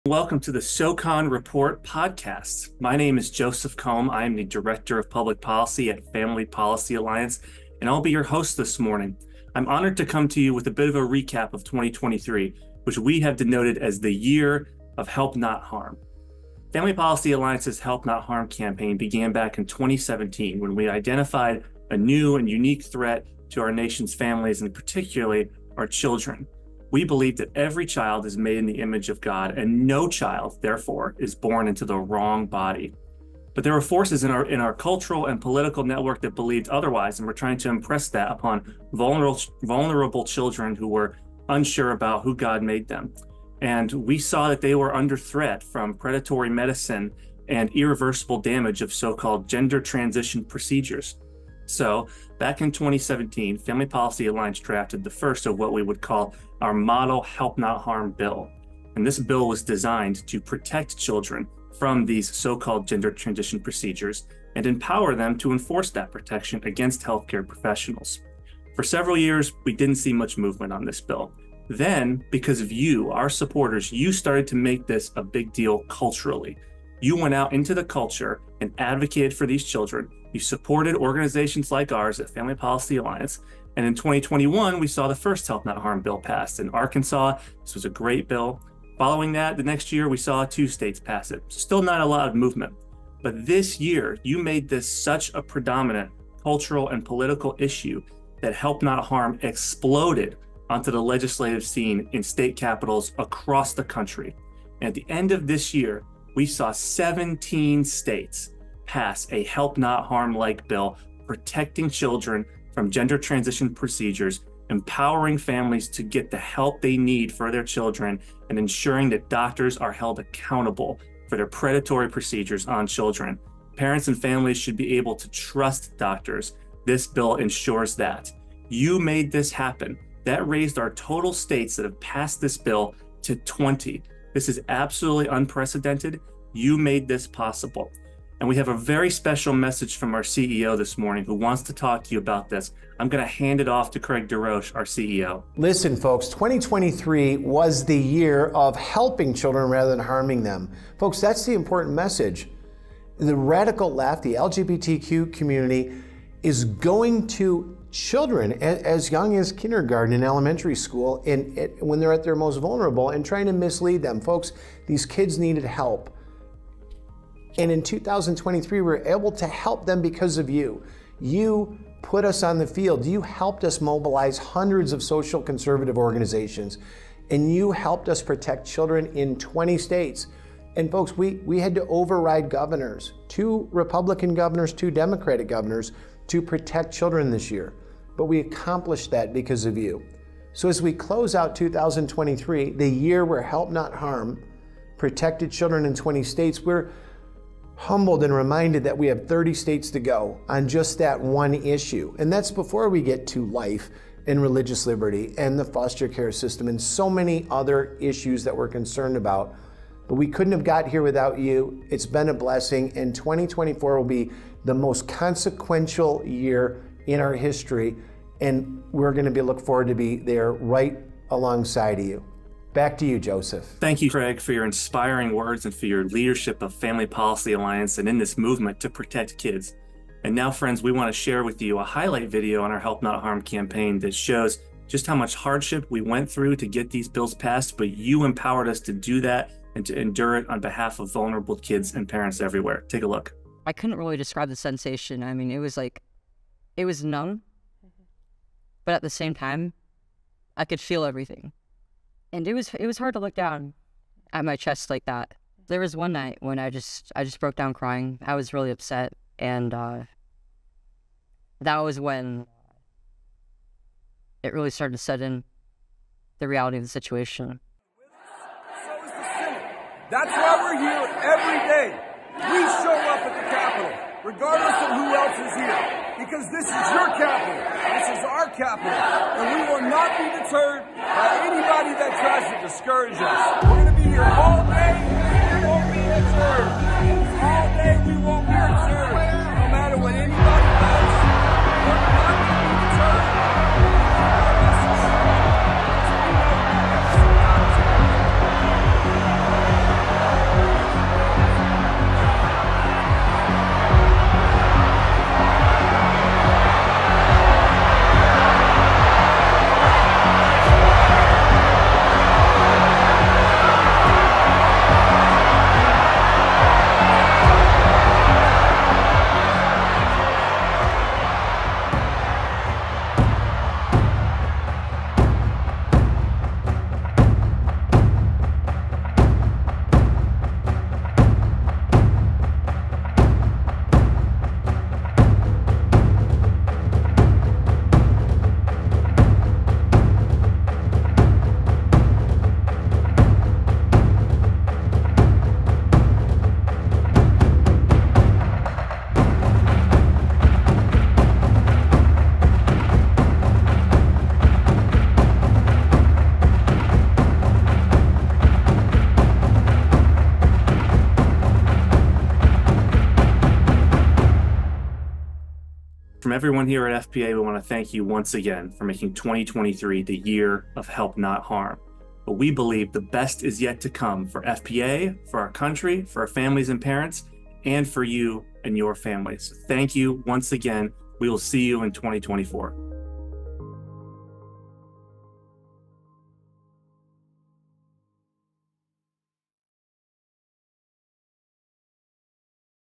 2023 Was the Year of HelpNotHarm!
This year, we achieved a significant milestone. Our Help Not Harm legislation was passed in 20 states to safeguard children from transgender drugs and surgeries. This means thousands of kids in those states are now protected from dangerous medical interventions!
And, we couldn’t have done it without YOUR partnership and support.

 2023 was truly the year of Help Not Harm! Let’s celebrate this achievement with a message from our CEO, Craig DeRoche, and an exciting highlight video that recaps out journey and how together we overcame obstacles to get where we are today. Tune in to watch the premiere online today!

At Family Policy Alliance, our vision is a nation where God is honored, religious freedom flourishes, families thrive, and life is cherished.